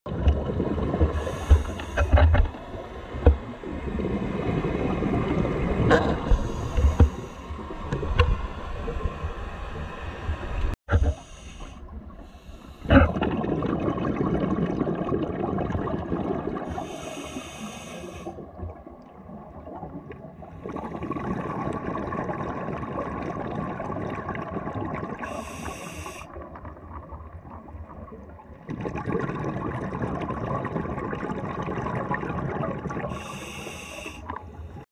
09 17 2021 scuba sculpture
While diving today at Windy Point Park Lake Travis Texas with Harry Ullmann, we encountered the scuba diver sculpture underwater.